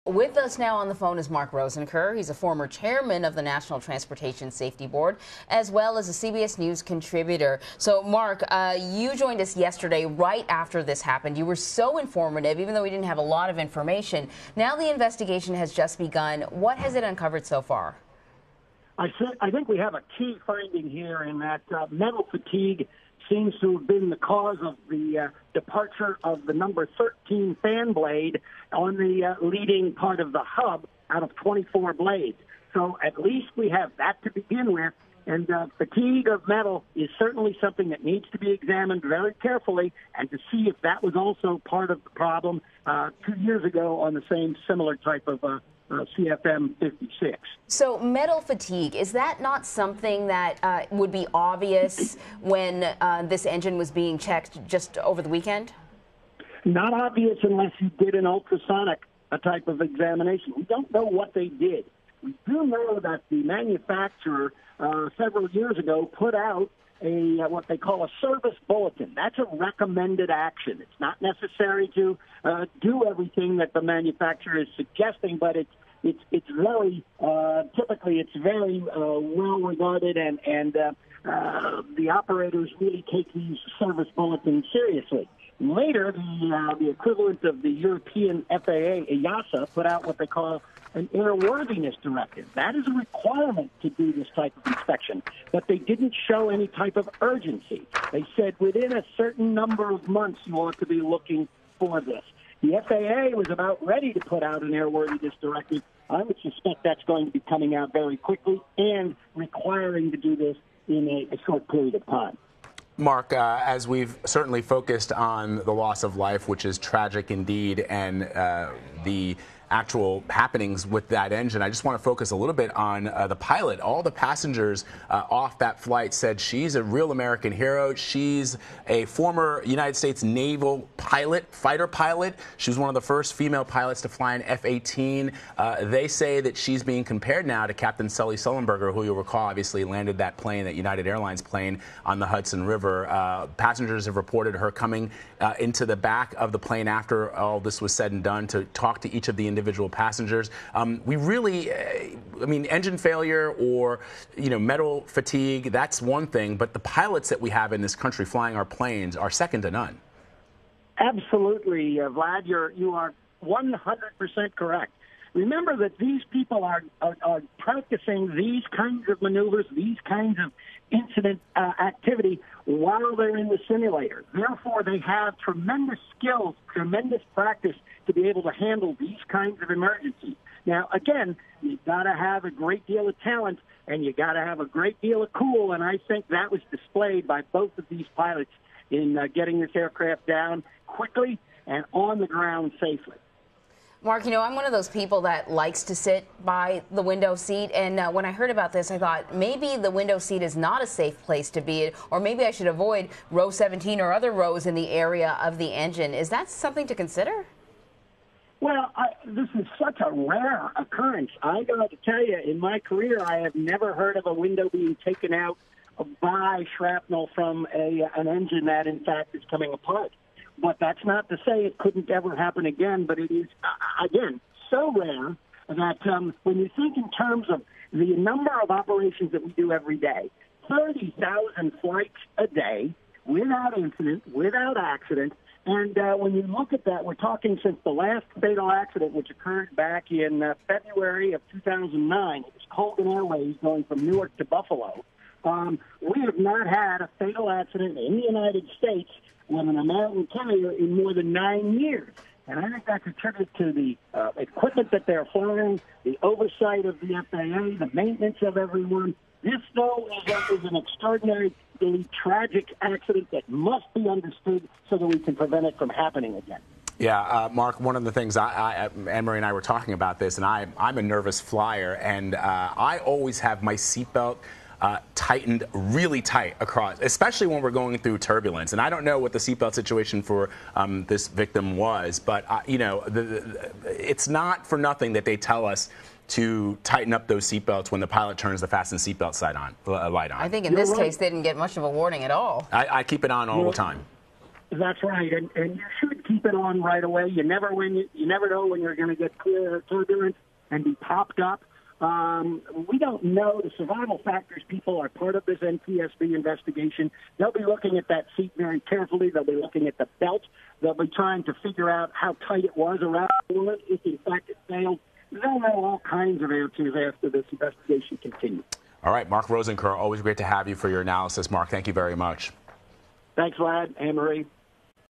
Former NTSB chairman explains explosion on Southwest Airlines plane
Yesterday's mid-air engine explosion on a Southwest Airlines plane is under investigation. One person was killed n the blast which may have been caused by metal fatigue in the engine's fan blades. Former NTSB chairman and CBS News contributor Mark Rosenker joins CBSN with more.